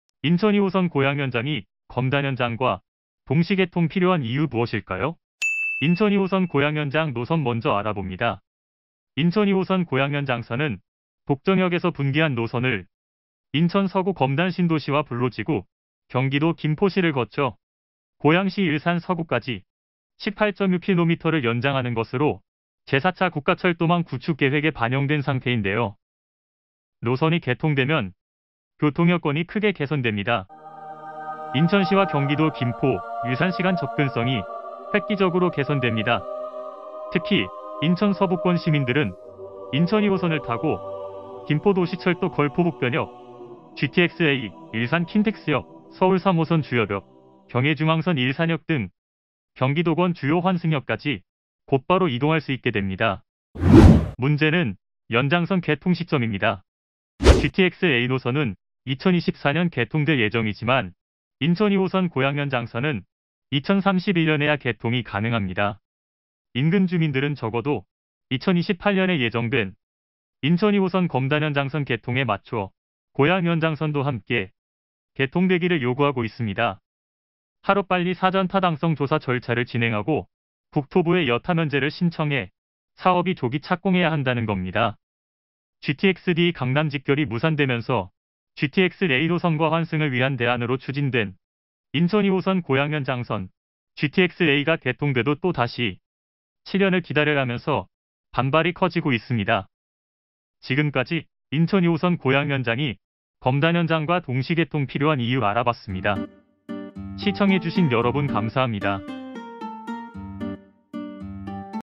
인천 2호선 고양연장 검단연장과 동시개통 필요한 이유? GTX A 일산 킨텍스역, 서울3호선 주엽역, 경의중앙선 일산역 환승해야
GTX A 노선은, 2024년 개통될 예정이지만
인천2호선 고양연장선은
2031년에야 개통이 가능합니다.

인근주민들은 적어도,
 2028년에 예정된
인천2호선 검단연장선 개통에 맞춰
 고양연장선도 함께
개통되기를 요구하고 있습니다.

인천2호선 고양연장이 검단연장과
동시개통 필요한 이유 무엇일까요?